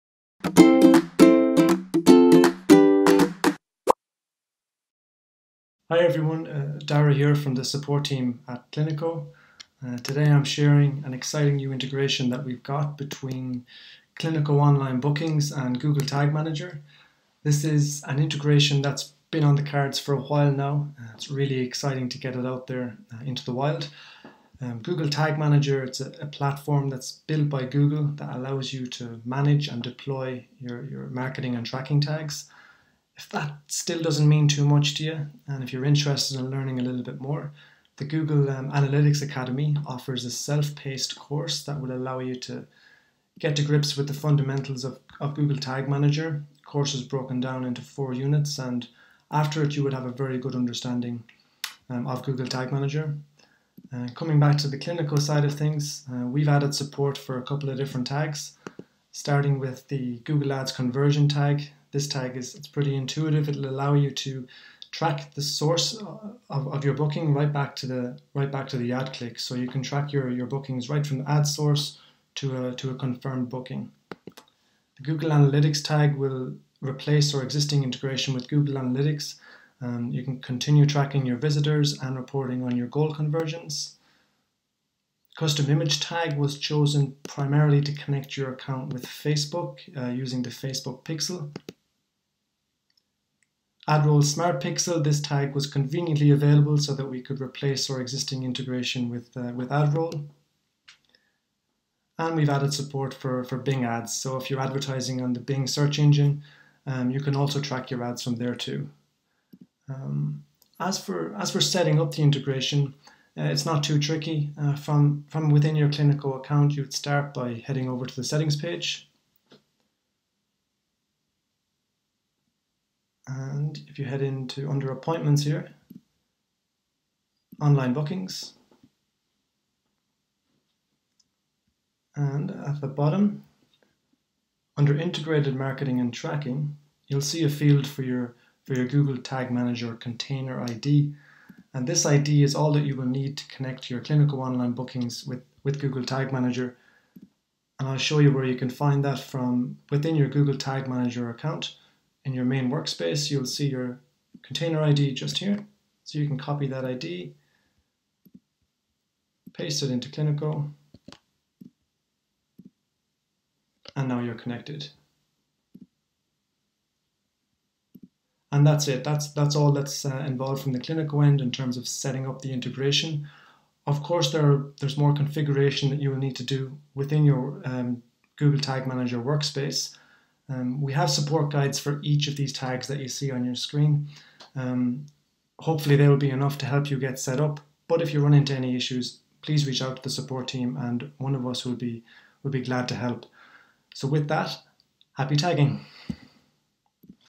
Integrate Google Tag Manager with your Cliniko online bookings
Use Google Tag Manager with your Cliniko online bookings to track website visitors, learn more about their online journey, and report on successful bookings!